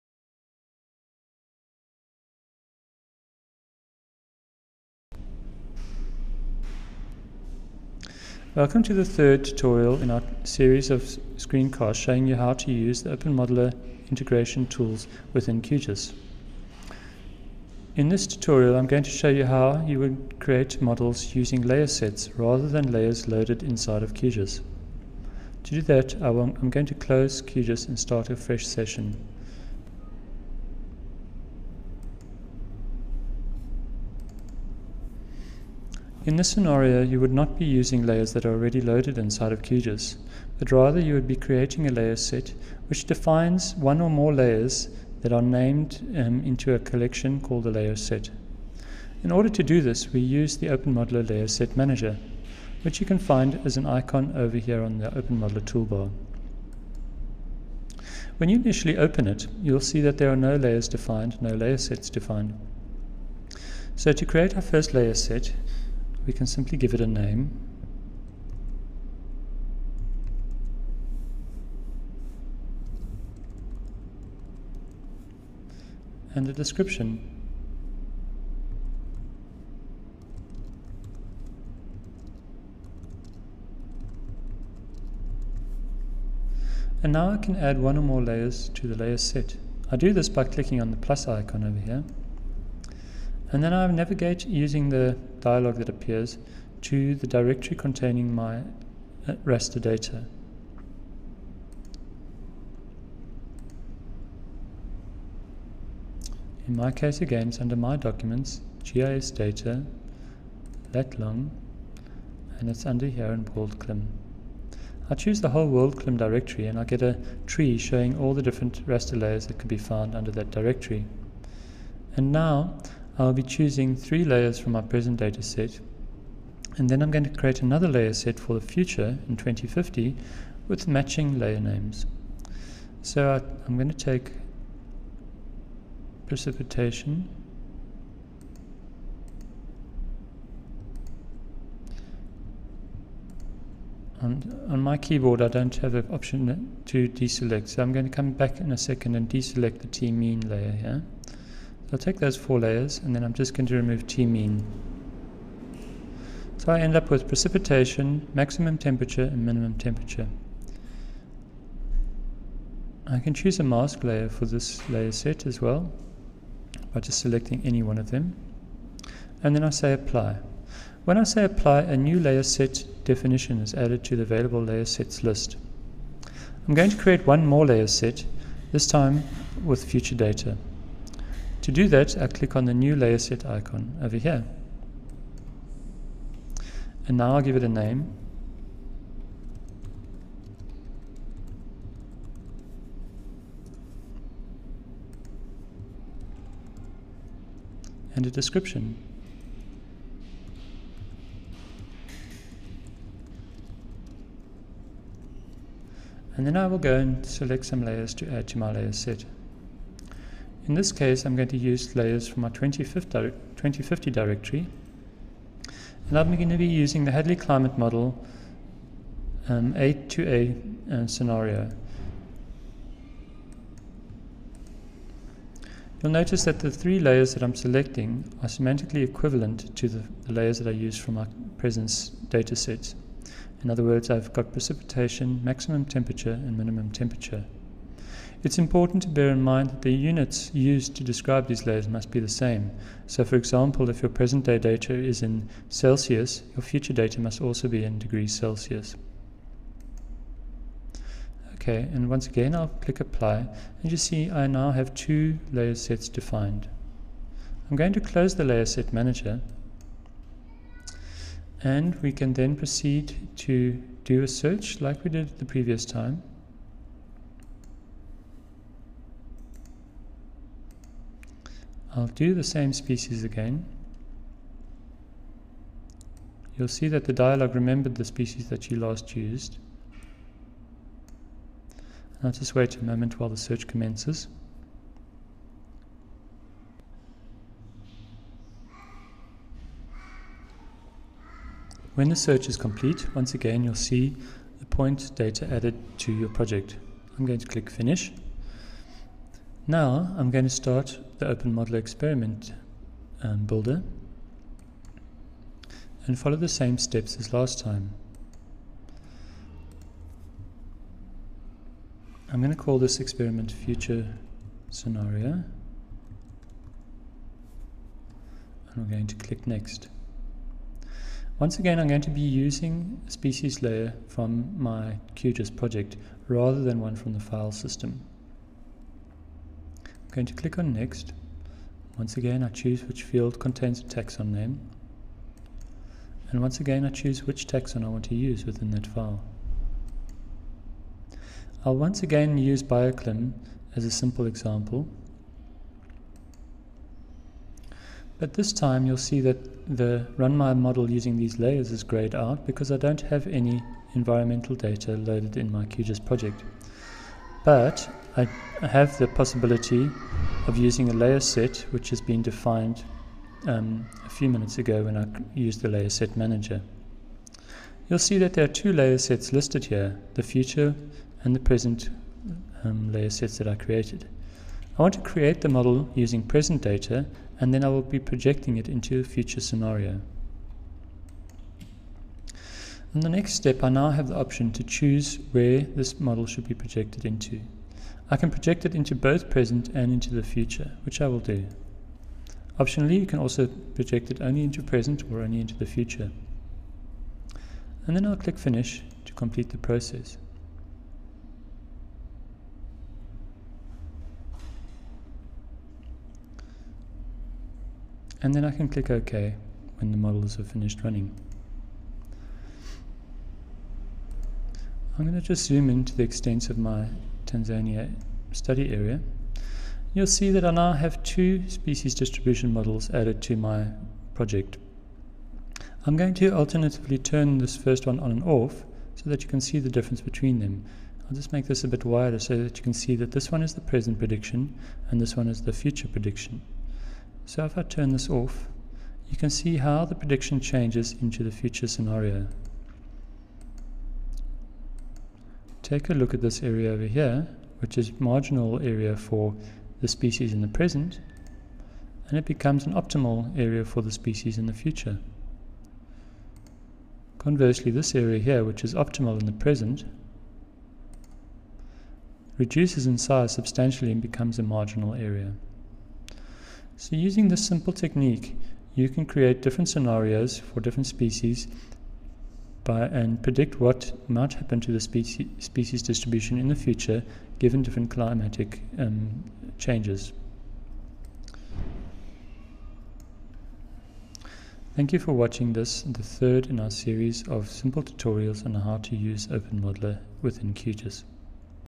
Projecting a model into a future climate scenario using the openModeller QGIS plugin
This video shows how to run a mode complex (procedurally) ecological niche model using the openModeller Desktop plugin that we have integrated into QGIS.